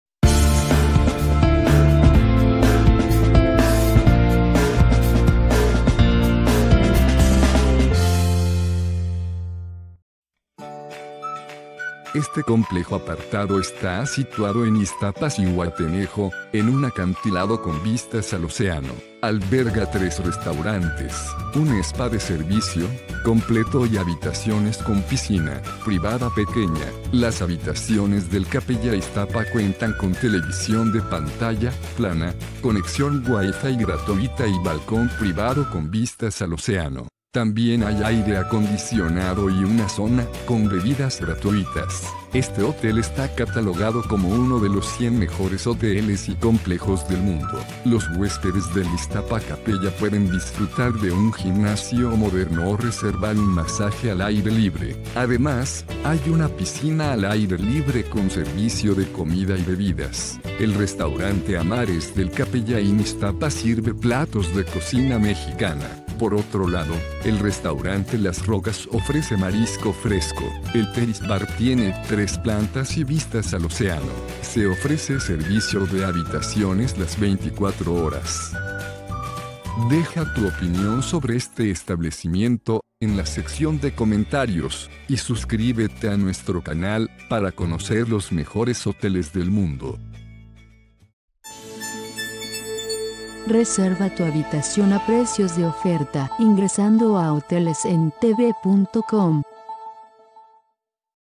Capella Ixtapa Resort & Spa
Capella Ixtapa Resort & Spa ofrece 53 habitaciones con minibar, caja fuerte y tv. La edificación de Capella Ixtapa Resort & Spa se encuentra localizada en una encantadora región de Ixtapa.